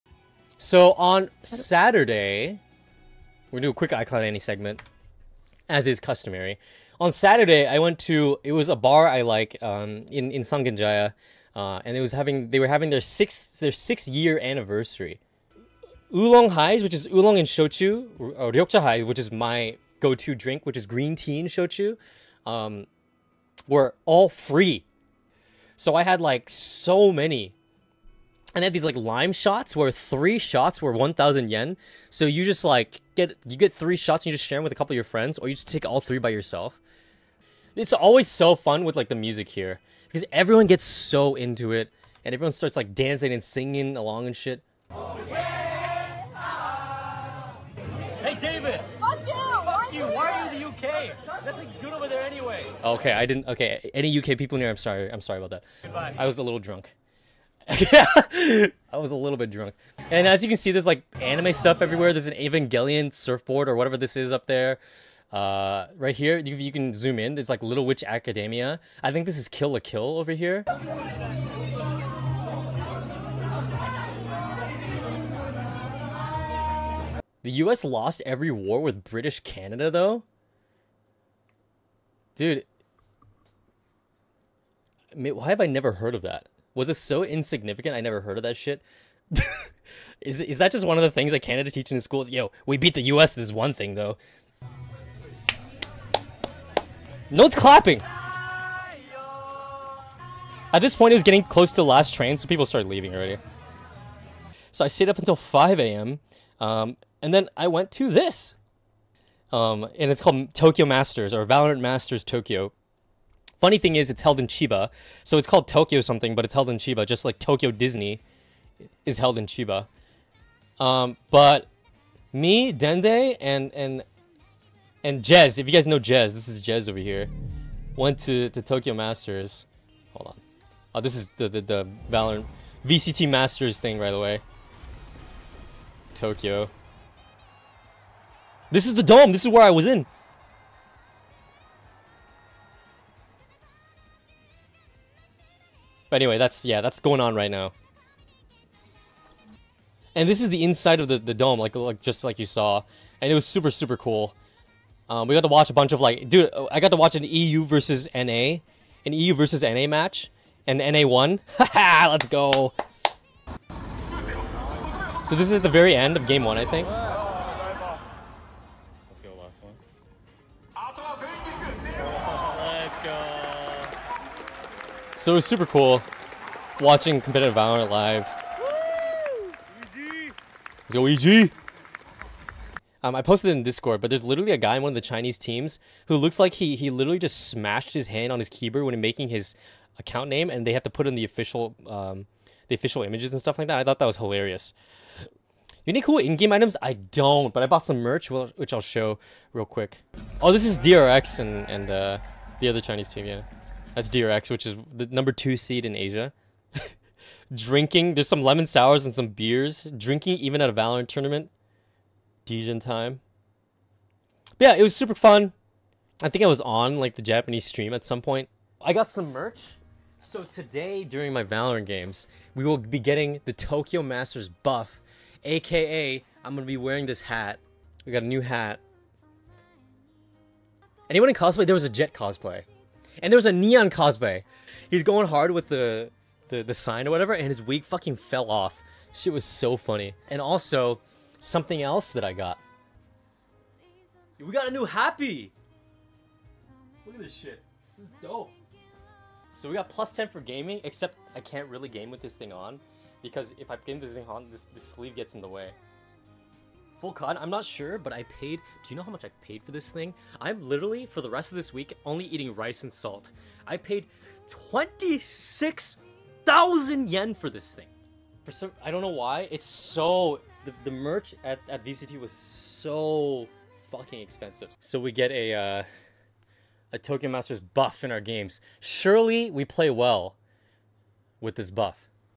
iCloud Andy (abridged version) - Life in Japan - June 13th 2023
Ellis went to a 6th anniversary of a bar and the newt day to Valorant Masters Tokyo event.

▶ SilleEllis's Twitch handle @silleellis
▶ SilleEllis's Twitter handle @SilleEllis63
▶ SilleEllis's Instagram handle @silleellis